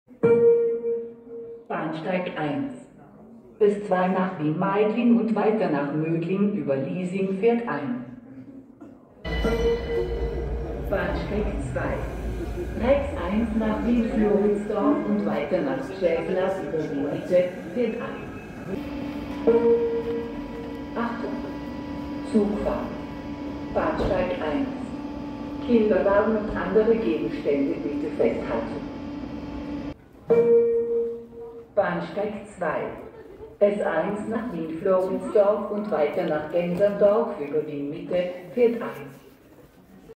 Hlášení na zastávce Wien Rennweg
Hlášení z podzemní zastávky vídeňského S-Bahnu, Rennweg.